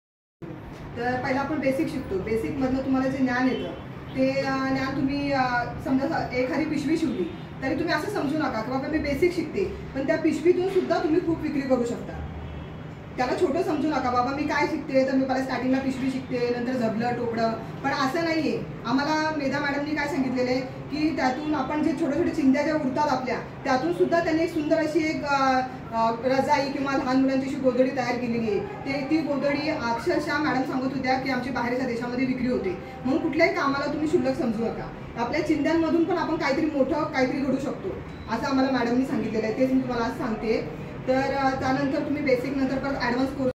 7.G - 20 JAN BHAGIDARI EVENTS & CONDUCTED “ENTREPRENEURIAL SLOGAN WRITING COMPETITION”
G - 20 JAN BHAGIDARI EVENTS & CONDUCTED “ENTREPRENEURIAL SLOGAN WRITING COMPETITION” & “EXPERT LECTURE BY INDUSTRY SPEAKERS” & STALL ACTIVITY ORGANIZED AT CHAUK-KHALAPUR & CHAUL -ALIBAG JAN SHIKSHAN SANSTHAN RAIGAD MAHARASHTRA CELEBRATE THE PROGRAMME UNDER G - 20 JAN BHAGIDARI EVENTS  & CONDUCTED “ENTREPRENEURIAL SLOGAN WRITING COMPETITION” & “EXPERT LECTURE BY INDUSTRY SPEAKERS” & STALL ACTIVITY ORGANIZED AT  CHAUK-KHALAPUR & CHAUL -ALIBAG IN PRESENCE OF  DR. VIJAY KOKANE (DIRECTOR), MRS. KALPANA MHATRE (ASSISTANT PROGRAM OFFICER), MRS. SIDDHI SALUNKE, MRS. MINAKSHI PAWAR (RESOURCE PERSON), MR. HIMANSHU BHALKAR (PROGRAMME ATTENDAT) & ALL STAFF MEMBER FROM JAN SHIKSHAN SANSTHAN RAIGAD & WOMEN BENEFICIARIES FROM CHAUK-KHALAPUR & CHAUL-ALIBAG RAIGAD ON DATE 09/06/2023 FRIDAY